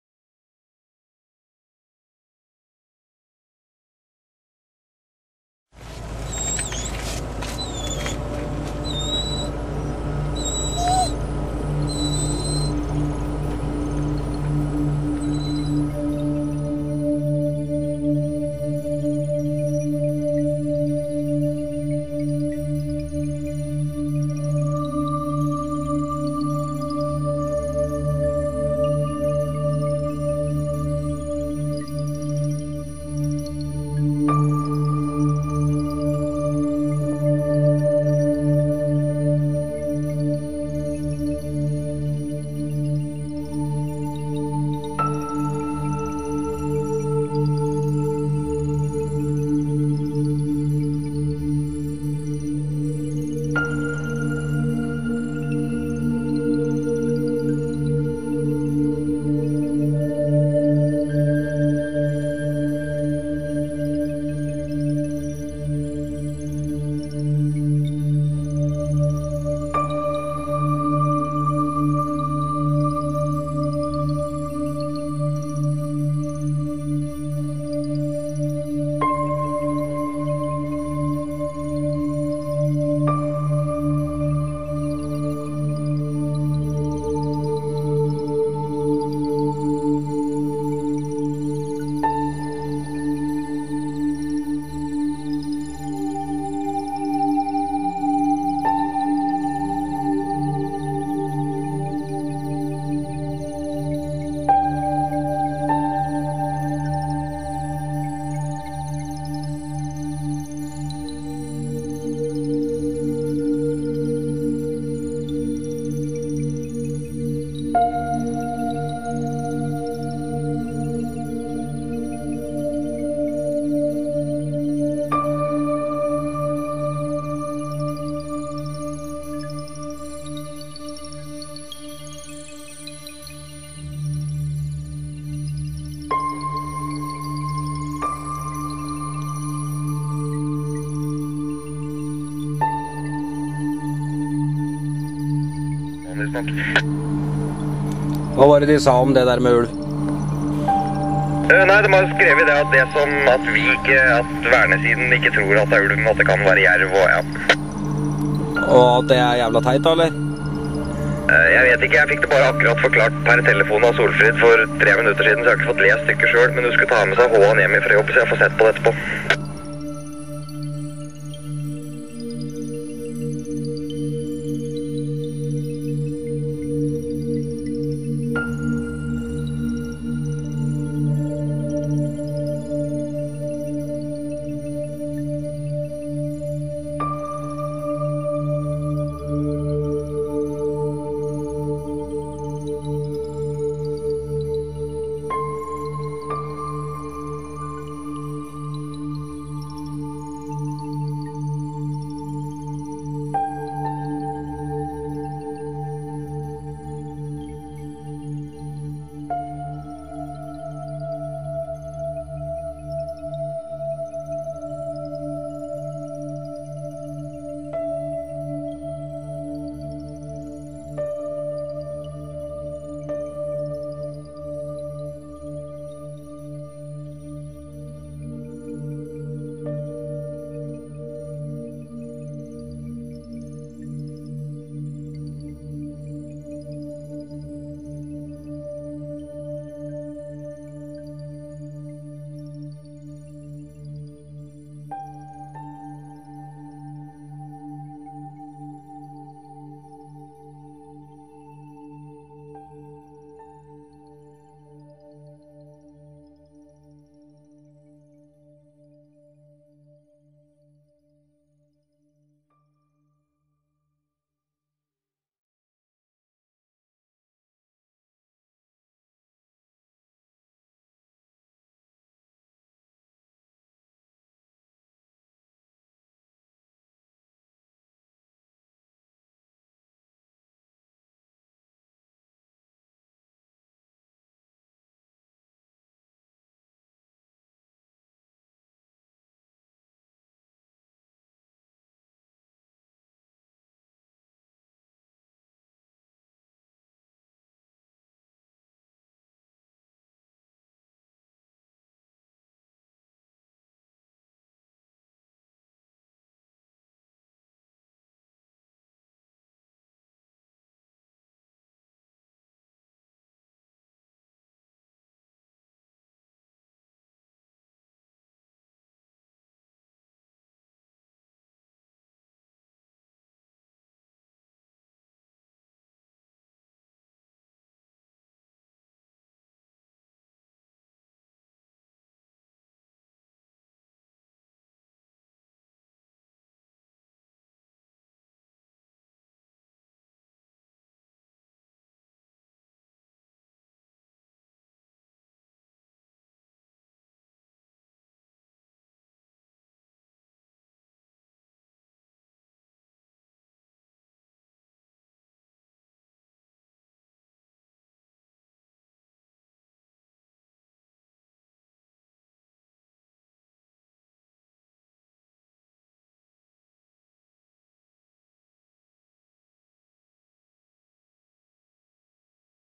Ulvejakt
Dette er noen klipp i fra Løten og Elverum, 2011 der et kommunalt jaktlag jaktet på en angivlig ulv.

Første klippet i videoen er i fra Slettås. Her blir en plakat revet ned i fra oppslagstavla, etter at det ble igangsatt en "kampanje" for å hevde at ulven i Slettåsreviret er farlige.